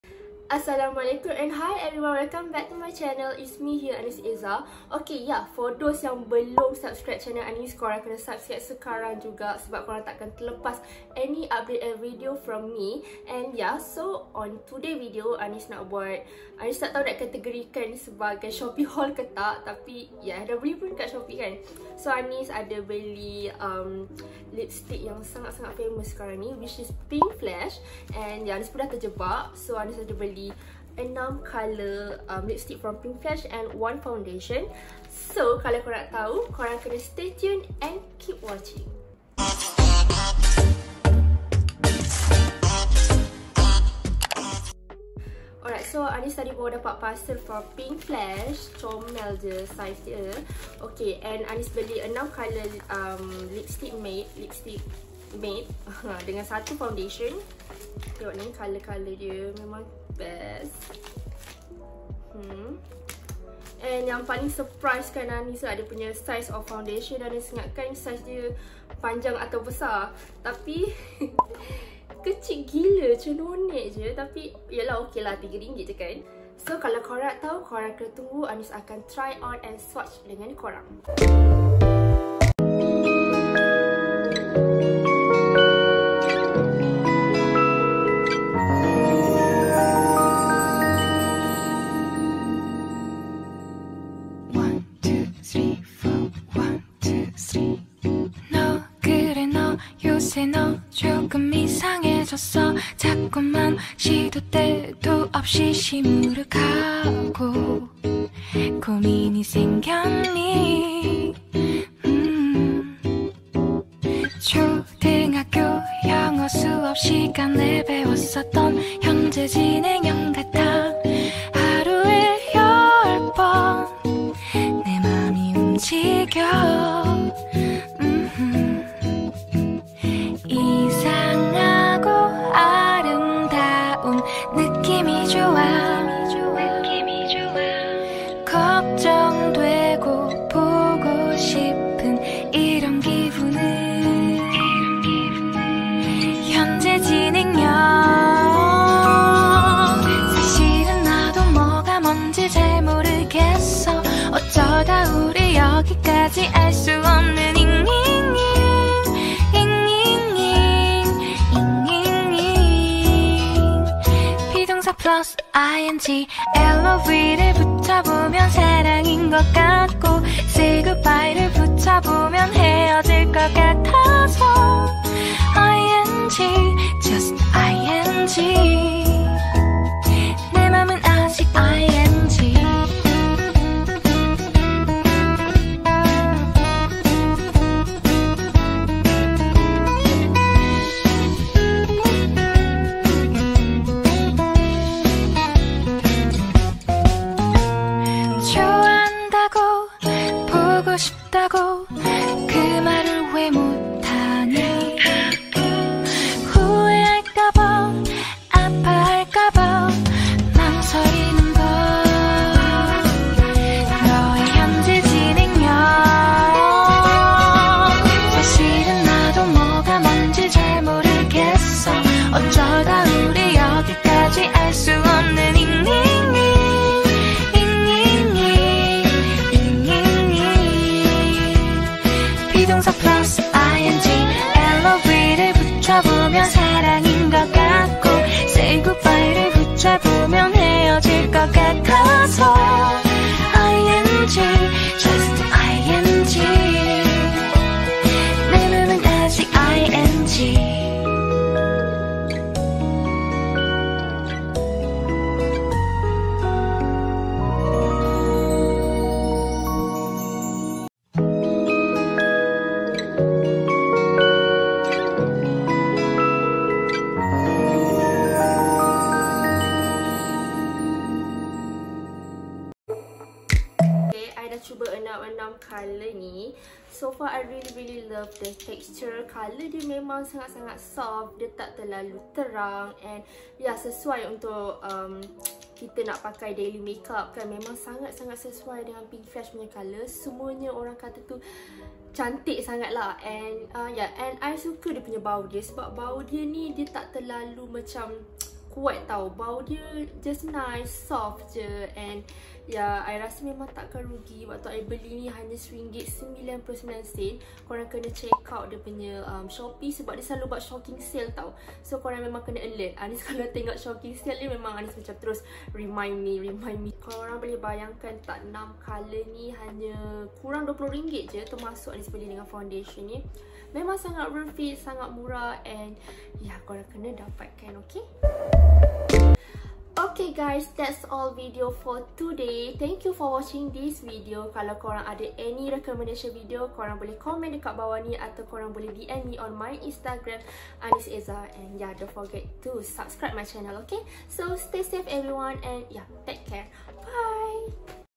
Shopee Haul Malaysia | Pink Flash Lipmatte 💄(Try On)
Assalamualaikum and hi everyone its me here Annis Ezza👸🏼 so this is my Shopee Haul for PinkFlash mmg  murah teruks🛍🛍

I just love this product so much❤️❤️